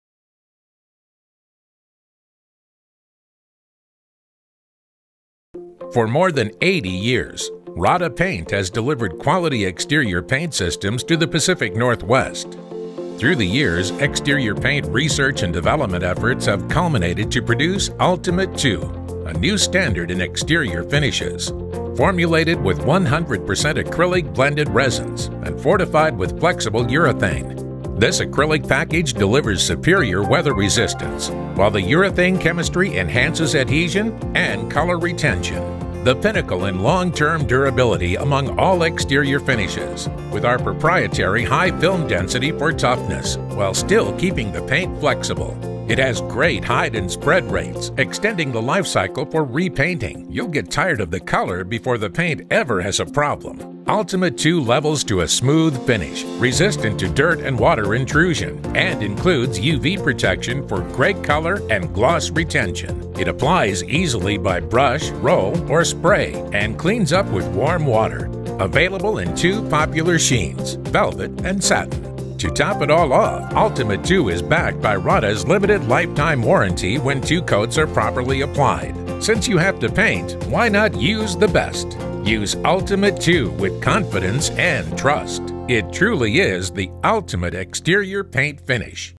Rodda Ultimate II Exterior Lifetime Paint
DescriptionRodda Paint’s Ultimate II line of exterior performance paints are formulated with advanced 100% acrylic resin technology fortified with urethane resin to provide excellent adhesion over most exterior surfaces, and still remains flexible in the changing temperature conditions of the Northwest. Ultimate II is easy to apply with minimal odor and dries to a beautiful uniform finish in two popular sheens and exhibits outstanding early-moisture and dirt pick-up resistance, resulting a cleaner long term finish. The acrylic formulation with advanced urethane resin delivers exceptional UV protection for better color and gloss retention, with additives to resist mold and mildew growth, Ultimate II extends the service life between repaints. Can be applied by brush, roller or spray down to 35°F and clean up
easily with water.
RECOMMENDED USES
Ultimate II is available in a flat and satin sheen and is an excellent choice for properly prepared exterior wood, siding, masonry, stucco, and primed metal using Rodda Paint’s premium First Coat primer, Surf Bond II or Control Primer. Ultimate II is a smart choice for new work and repainting prestigious residential and commercial projects, or whenever long-term service life and added cost-per-year value is desired. Ultimate II Series is available in one gallon cans and five gallon pails and is backed by the Rodda Paint Warranty on commercial applications and residential applications (see your Rodda Sales Rep or Store Manager for details), and is supported by 1320+ colors in Rodda’s “Color Right” system, the exclusive Cascadia colors of the Northwest neighborhoods and can be custom-matched to most any color. Ultimate II – truly the ultimate exterior paint system. Vinyl siding should only be repainted with light colors to help avoid warping, see and follow siding manufacturer’s recommendations.